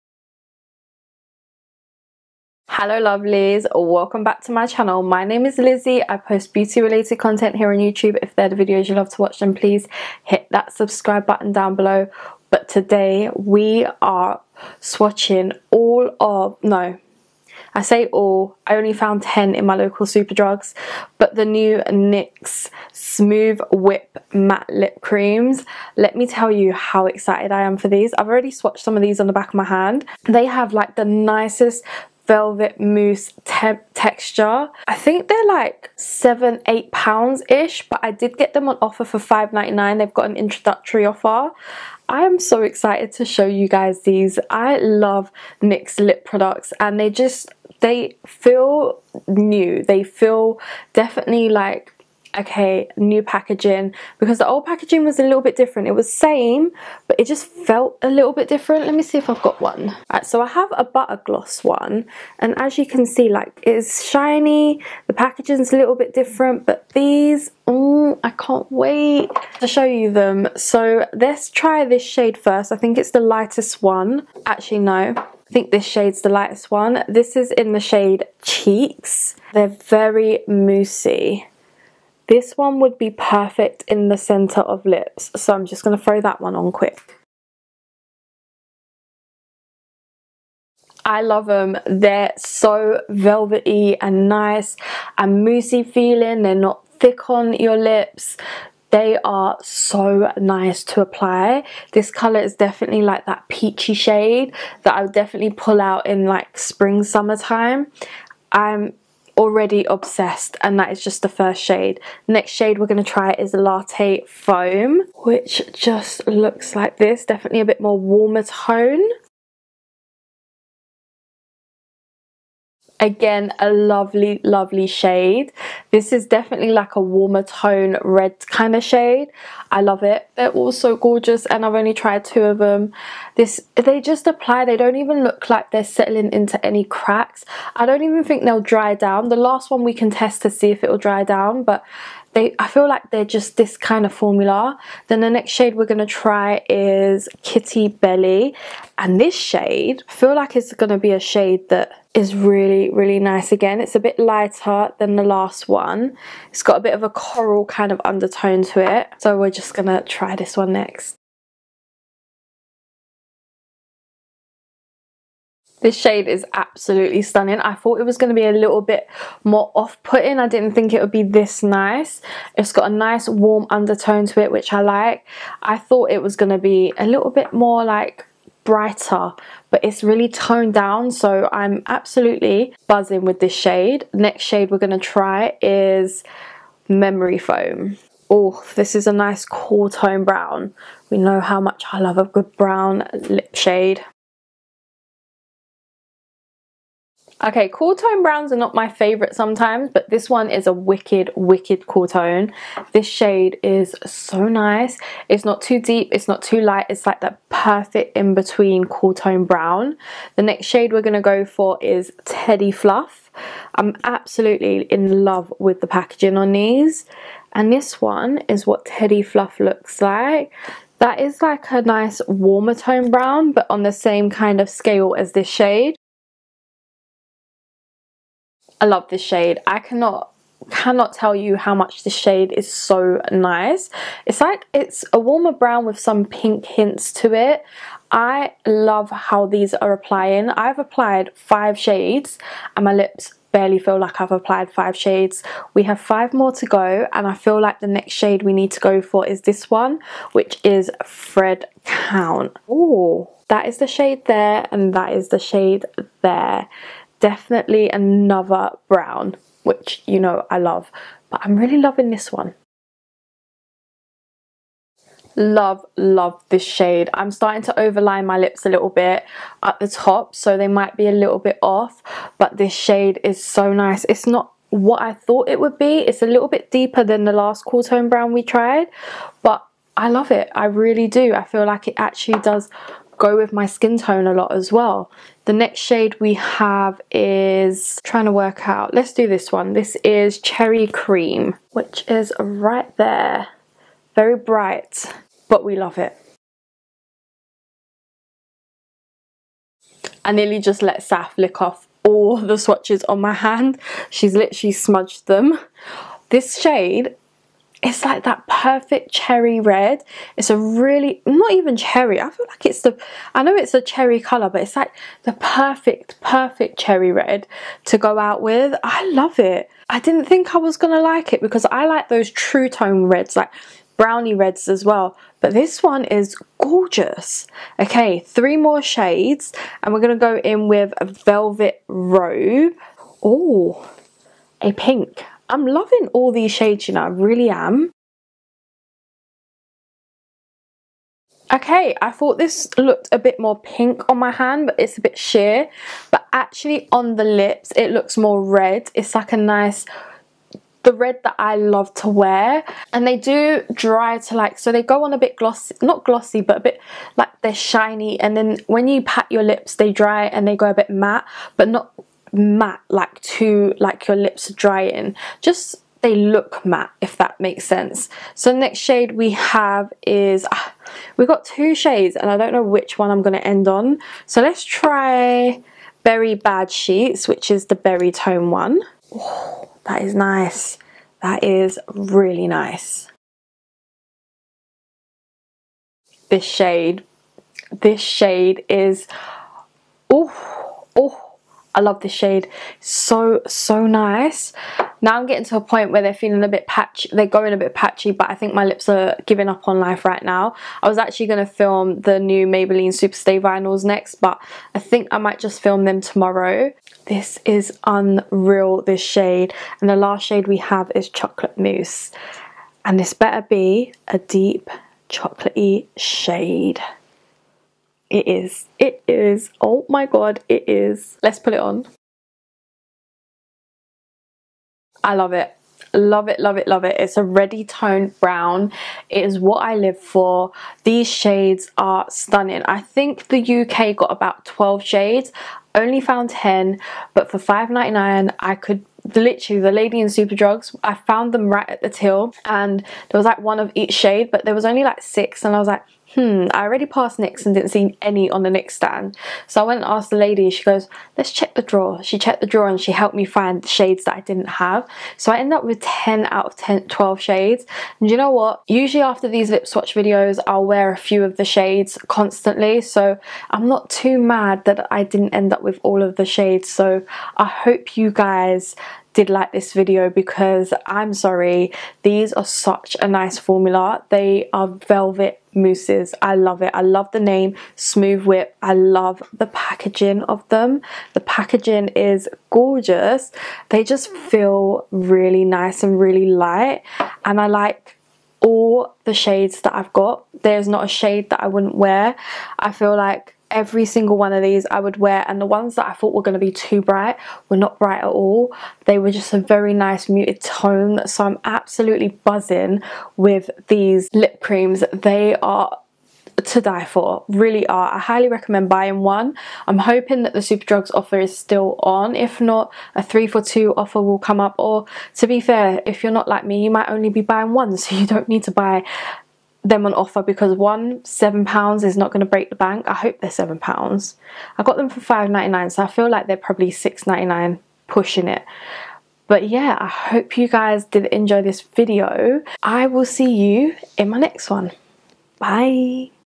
NYX MAKEUP SMOOTH WHIP MATTE LIP CREAM LIP SWATCHES | LIPSTICK LIZZIE
In todays video we are swatching the NYX Whip Lip Creams, I hope you enjoy.

*Dark chocolate brownie palette was kindly gifted to me from Revolution but this does not alter my opinion in anyway